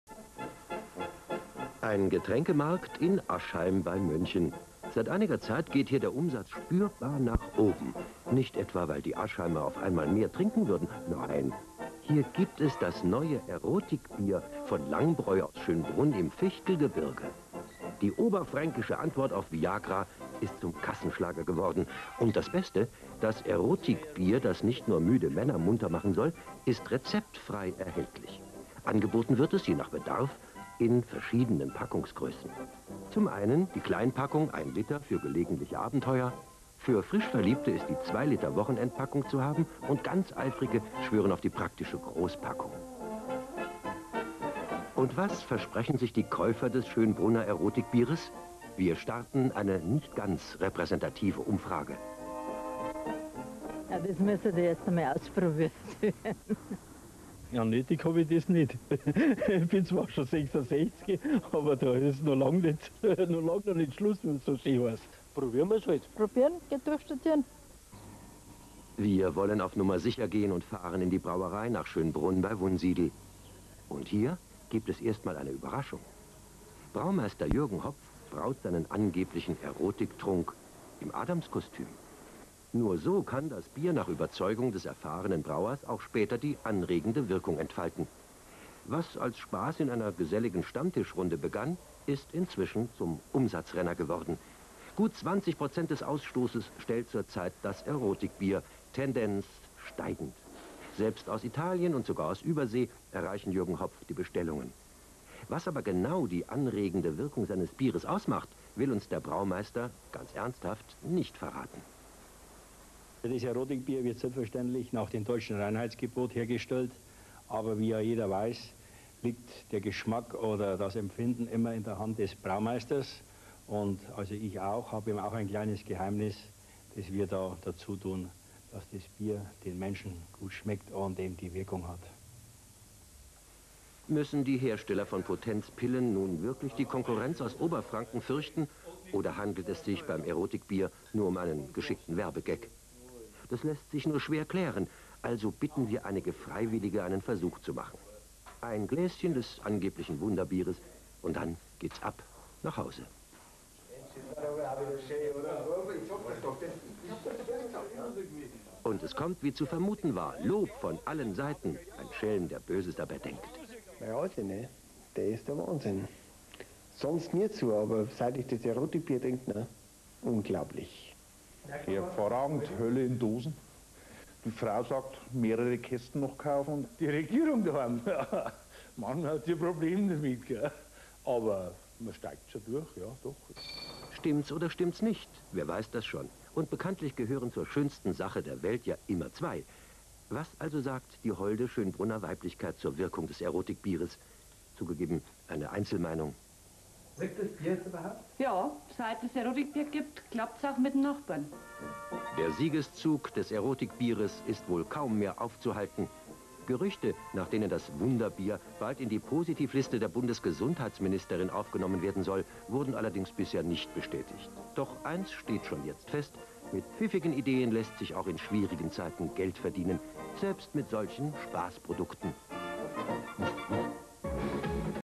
Lang-Bräu Erotikbier im Bayer. Fernsehen
Eine Reportage des BR über unser Erotikbier - das Bier, dass rezeptfrei müde Männer wieder "munter" macht.